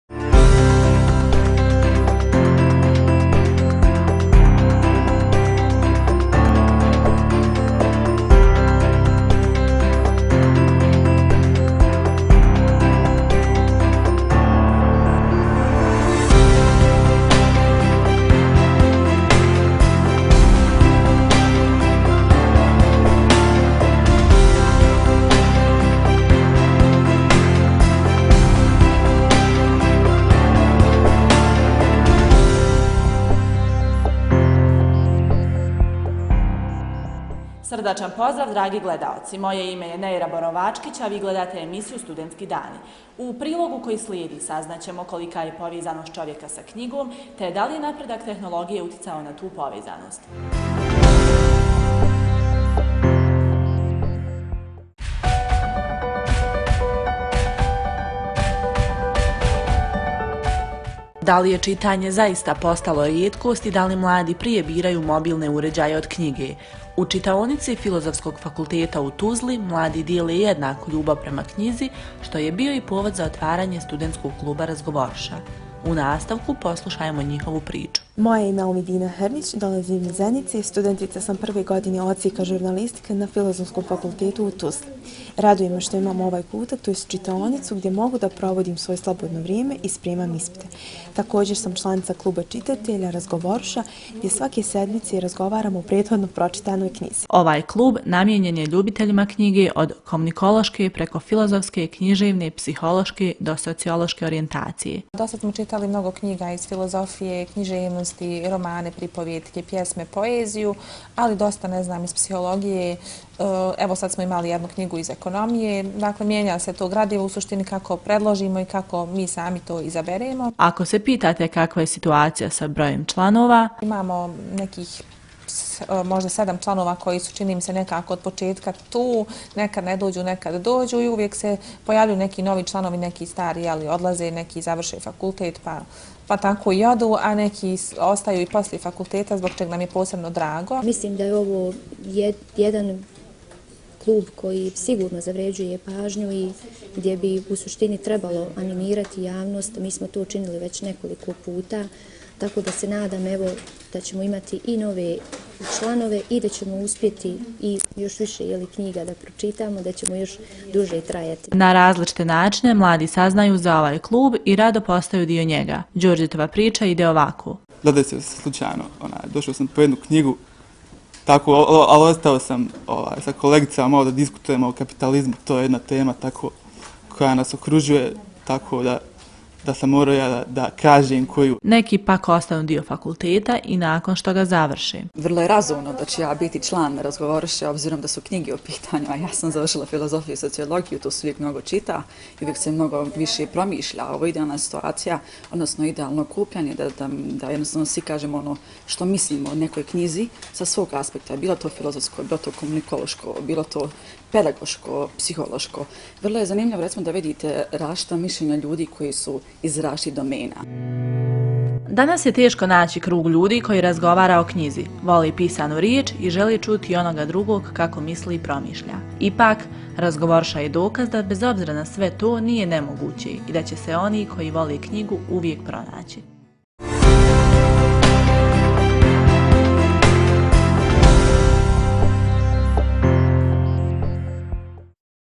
'Razgovoruša' jedinstven klub ljubitelja knjige u Tuzli!
Da li je čitanje zaista postalo rijetkost i da li mladi biraju mobilne uređaje prije knjige? U čitaonici Filozofskog fakulteta u Tuzli mladi dijele jednaku ljubav prema knjizi što je bio i povod za otvaranje studentskog kluba 'Razgovoruša'. Ovo je njihova priča...